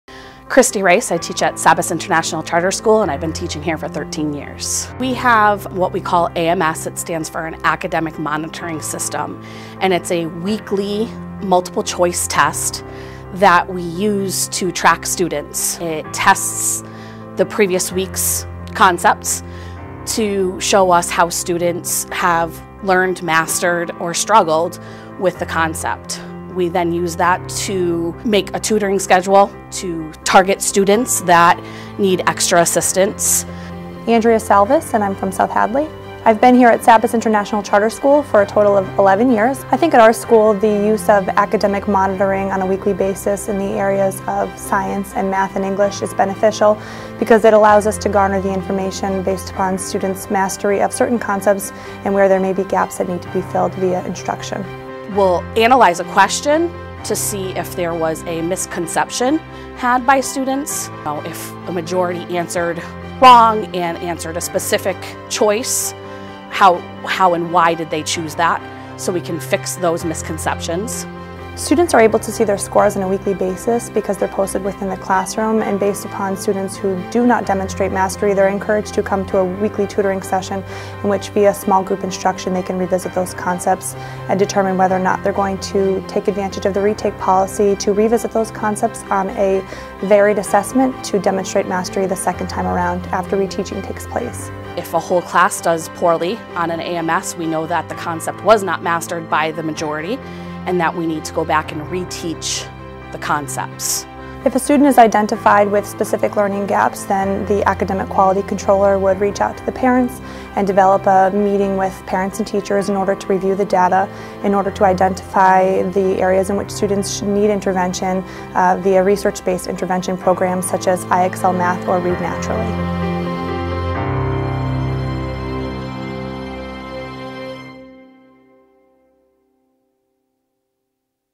Best Practices: How SABIS Springfield Charter Uses Data to Help Students Succeed
Teachers from SABIS International Charter School in Springfield  share strategies to improve academic outcomes for a student population that is mostly low-income and minority. Learn more about how Massachusetts charter schools are helping our neediest students succeed!